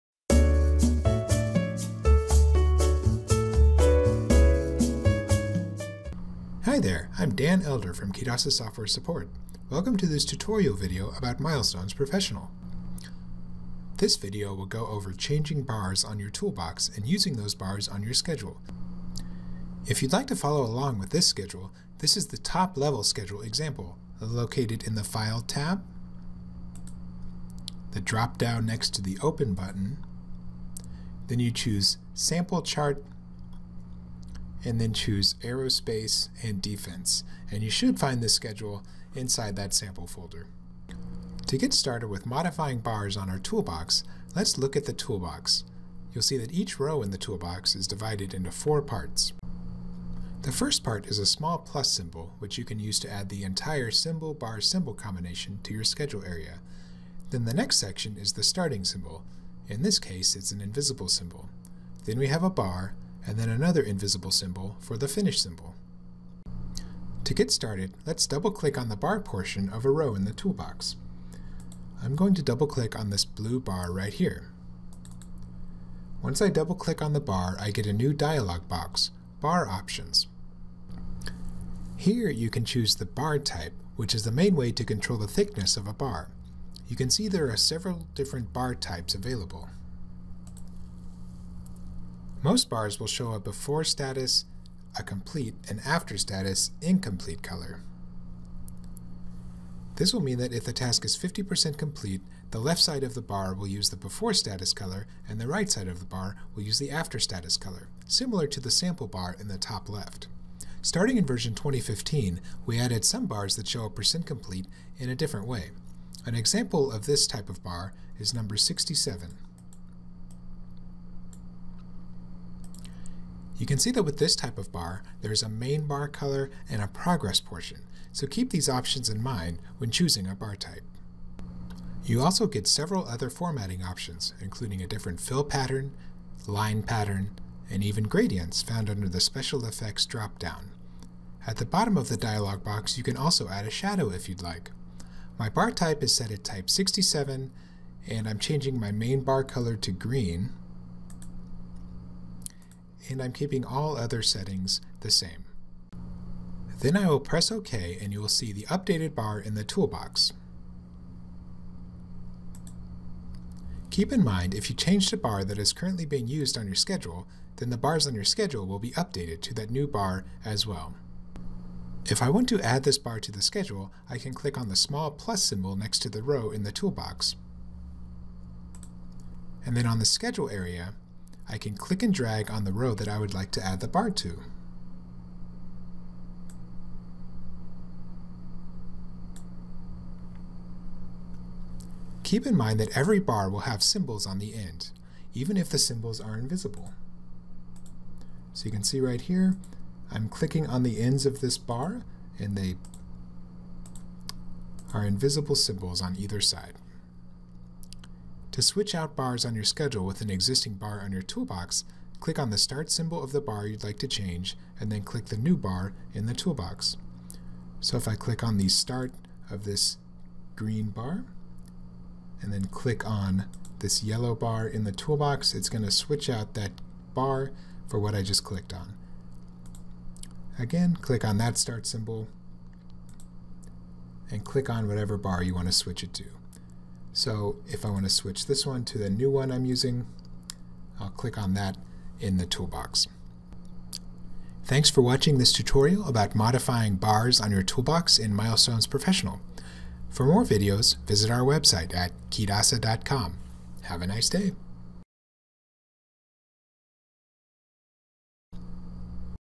Milestones Professional Tutorial - Change Bars on the Toolbox
Learn how to modify bars on your Toolbox in Milestones Professional.